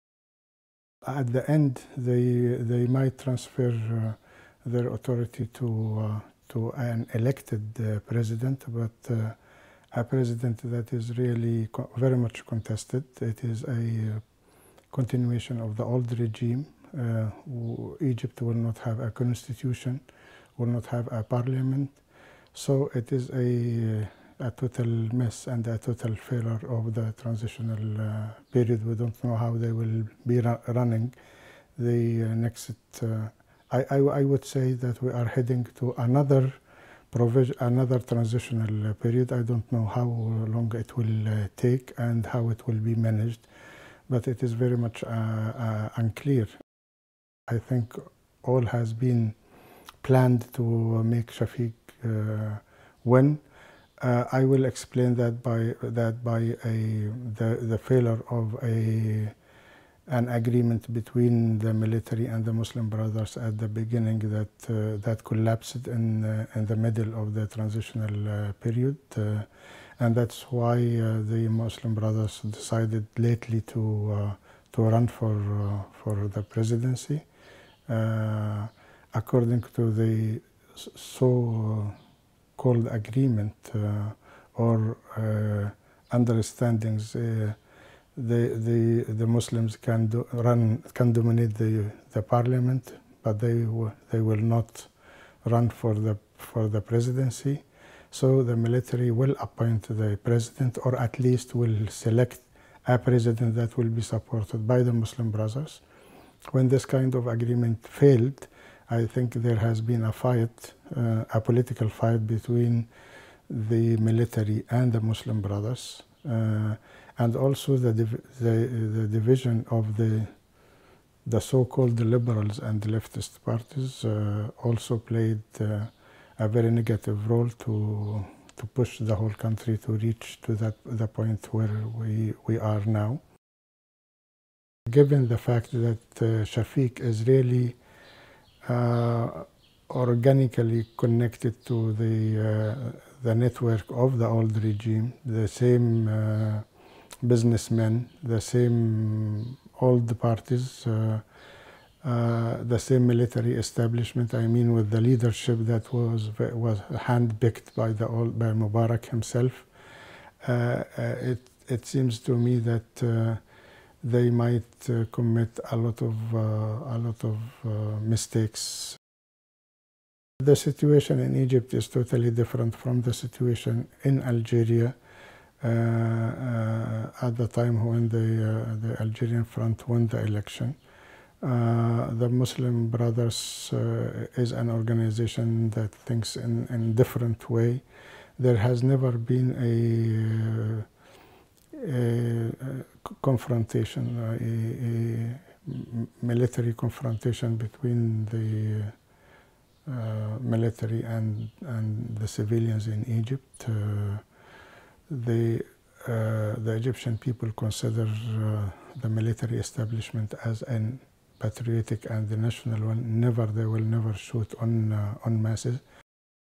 Professor Hassan Nafaa - Egyptian Politics
Author and political scientist at Cairo University, Professor Nafaa provides his analysis of the Egyptian presidential election and the political climate of the country. Highlights of the interview - conducted while the runoff election was taking place